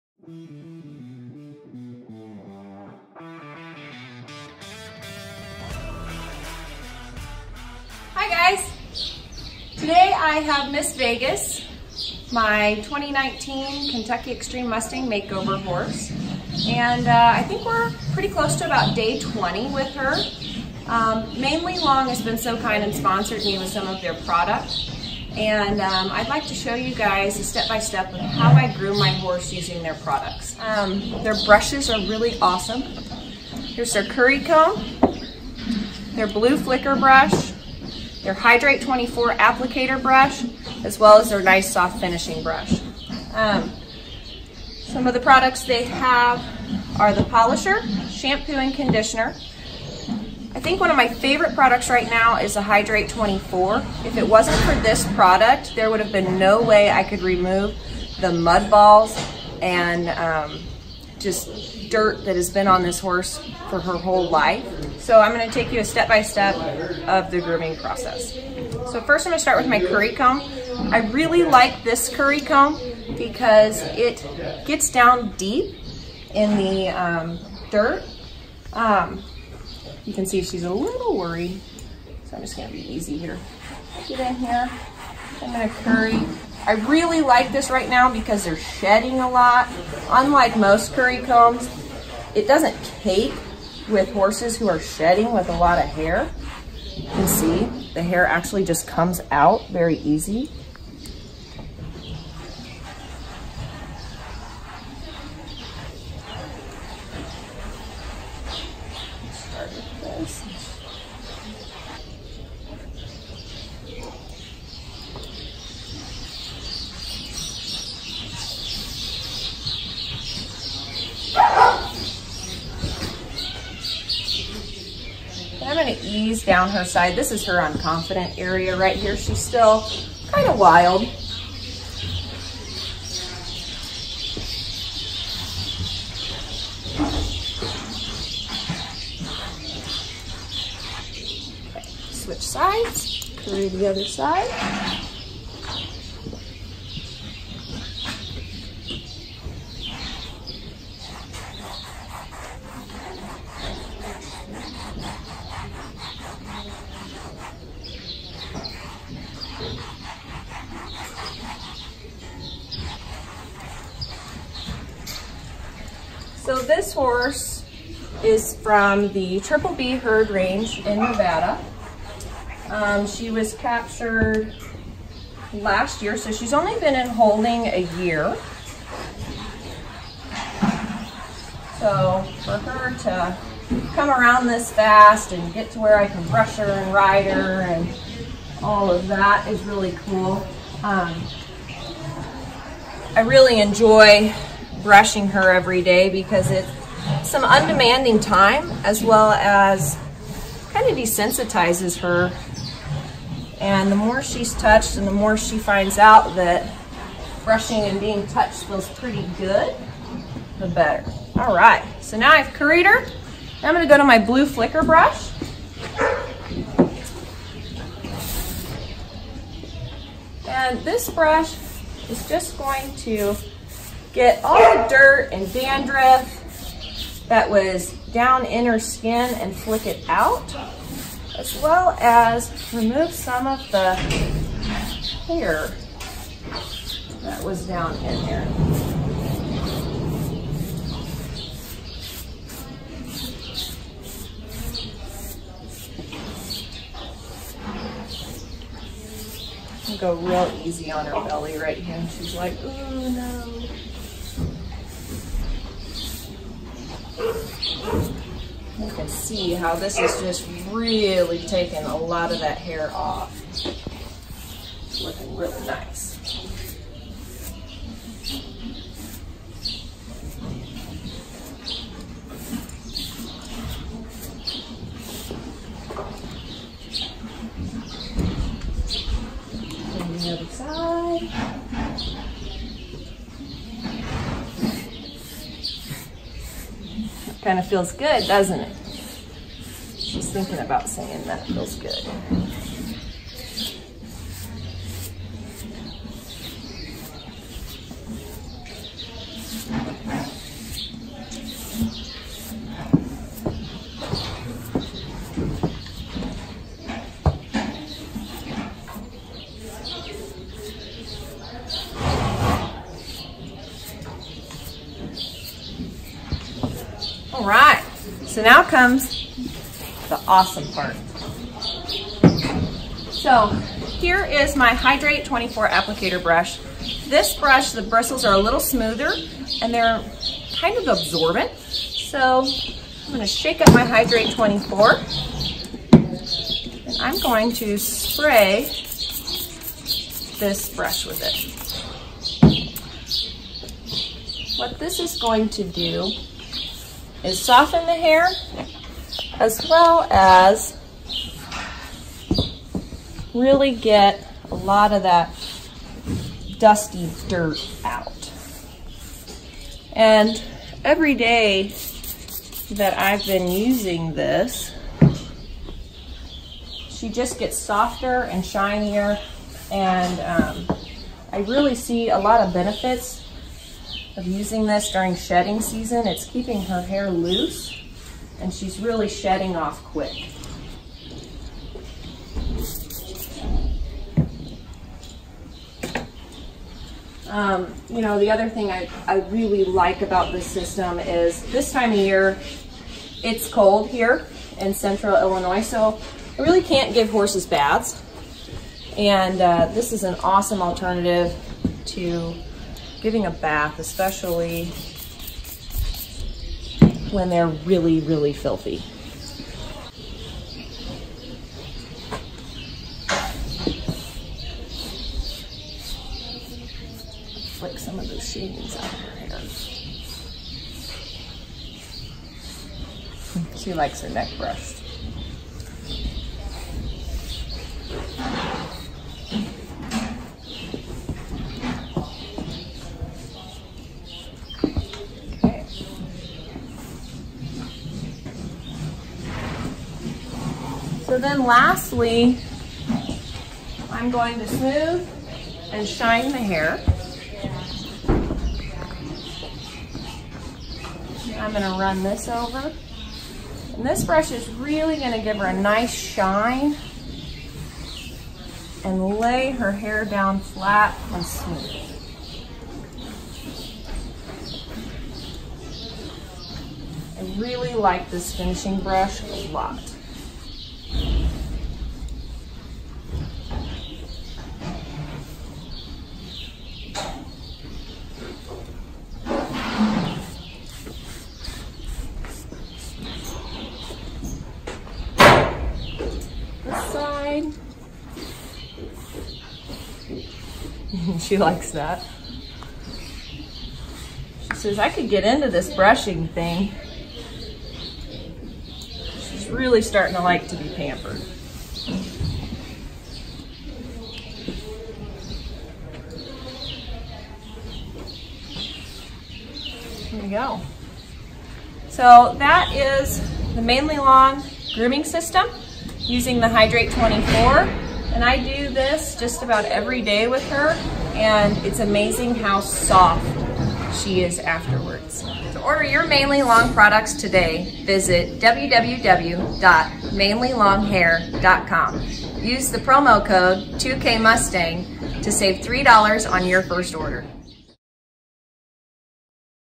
Mane ly Long Hair complete grooming system using Hydrate 24
Kaylee Kapraun demonstrates how to use the Mane-ly Long Grooming products on her 2019 Kentucky Extreme Mustang Makeover Horse, Vegas.
Hydrate 24 is a very special mixture that we teach you to make using our MLH Conditioner and H-24 Spray Bottle System for outstanding results! The bottles allow you to Mix the concentrated product properly so it will psray and give the desired results. Use without the bottles simply is NOT Hydrate-24- we treid for years to make this available at the lowest cost while keeping it easy easy easy to use!